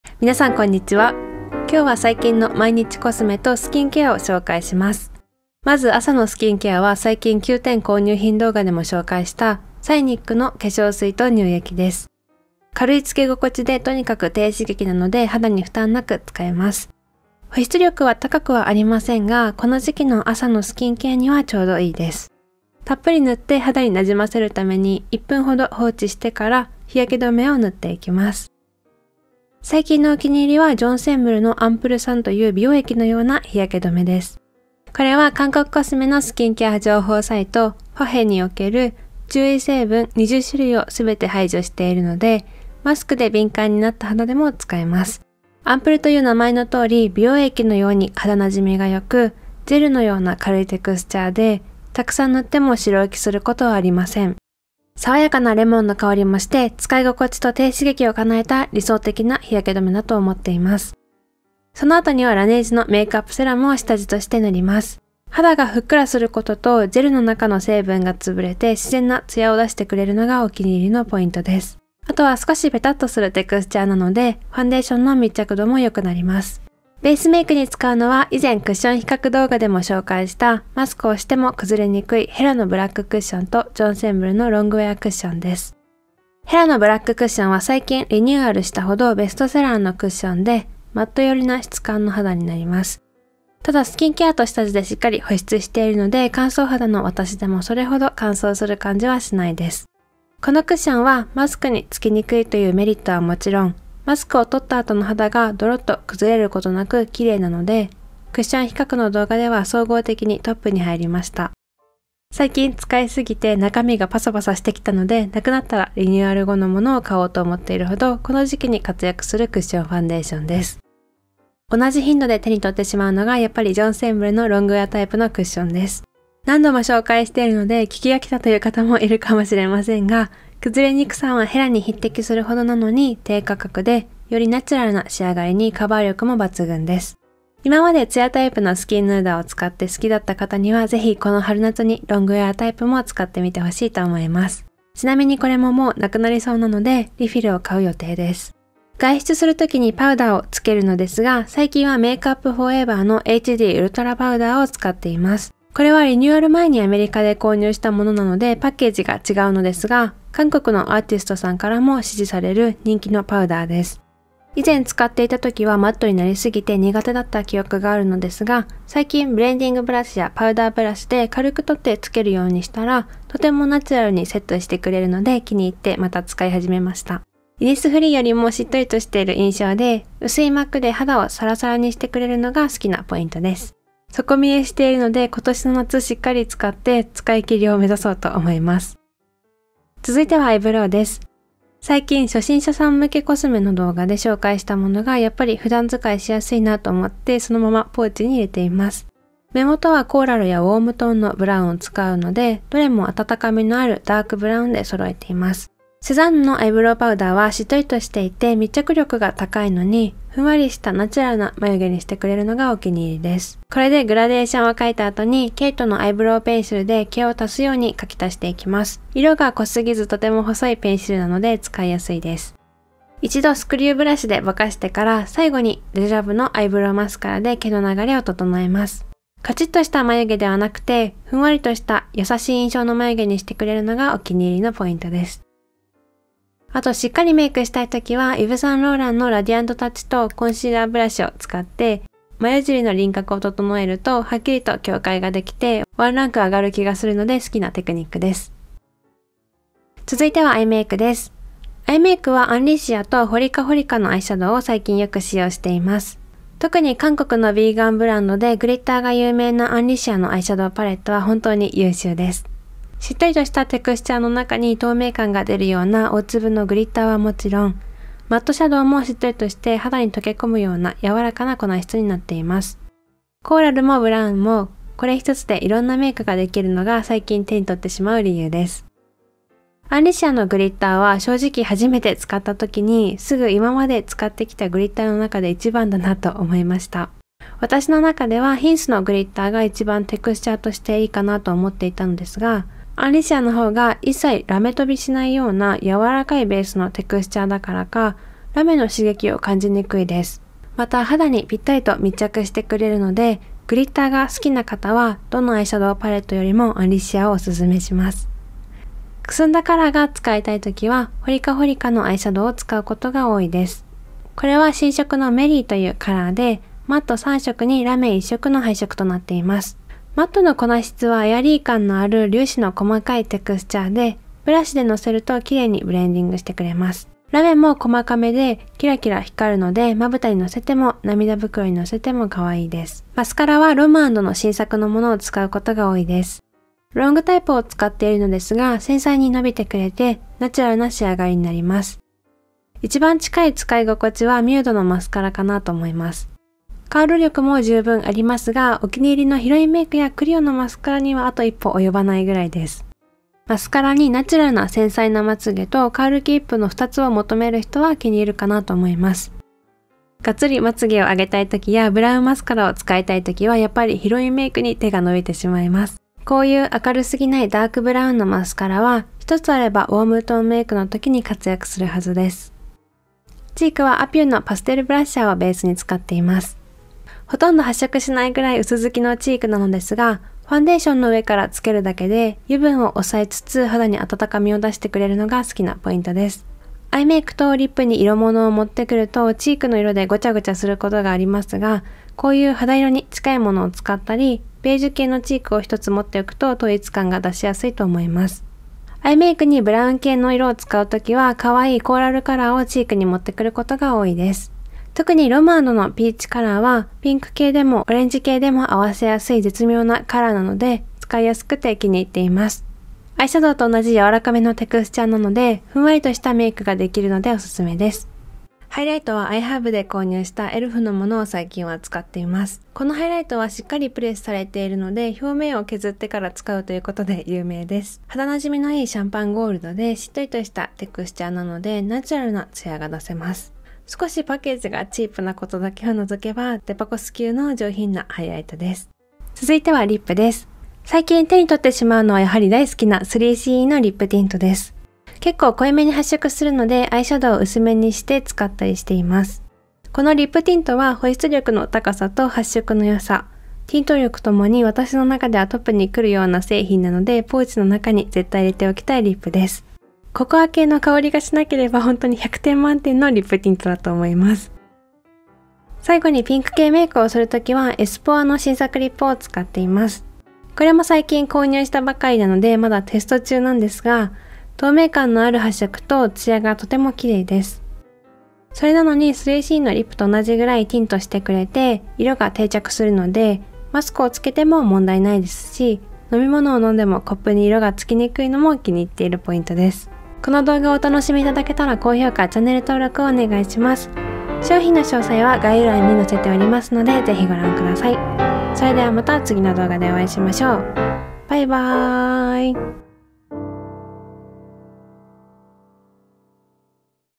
【毎日メイク 2021】最新韓国コスメでナチュラル春メイク！【詳細レビュー】
今日は最近の春メイクを意識した毎日メイクを紹介します！
最新の韓国コスメも使用しているので参考にして頂けたら嬉しいです☺️

使用アイテム:

ポーチ Aesopでセットを購入した時についてきた非売品です😭

Scinic ザシンプル カーミングトナー　カーミングローション ¥1919

*ジョンセンムル マスタークラスアンプルサン ¥3500

Laneige ラネージュ メイクアップグローイーセラム ¥2190

ジョンセンムル エッセンシャルスキンヌーダーロングウェアクッション Light ¥2630

メイクアップフォーエバー ウルトラHDプレストパウダー ¥4950

セザンヌ ノーズ＆アイブロウパウダー 01 キャメル ¥638

ケイト アイブロウペンシルA BR-3 自然な茶色 ¥605

デジャヴュ フィルム眉カラーアイブロウカラー ナチュラルブラウン ¥880

イヴ・サンローラン ラディアント タッチ 02 ルミナスアイボリー ¥5500

*アンリシア グリッターペディア 02 03 ¥2950

*ホリカホリカ マイフェイブアイシャドウパレット Merry ¥1470

ロムアンド ハンオールフィックスマスカラ LO1 ロングブラック ¥1490

ヒロインメイク ロング＆カールマスカラスーパーWP 02 ¥1100

アピュー　パステルブラッシュ CR02 ¥935

ロムアンド ベターザンチーク 02 ¥1199

E.L.F. ベイクド・ハイライター ムーンライト ¥815

3CE グレイズリップティント Any better ¥1610円

Espoir クチュールリップティントシャイン チリングチリング ¥1584

►ネイル
Essie Groom servise / Apres-chic

►使用機材
カメラ iPhone12
編集 Final Cut Pro

この動画はスポンサー提供ではありません。今後のコンテンツ制作の助けになるので、宜しければご利用ください。*マークのついているものはブランド又は小売店よりPRギフトとして商品提供していただいたものです。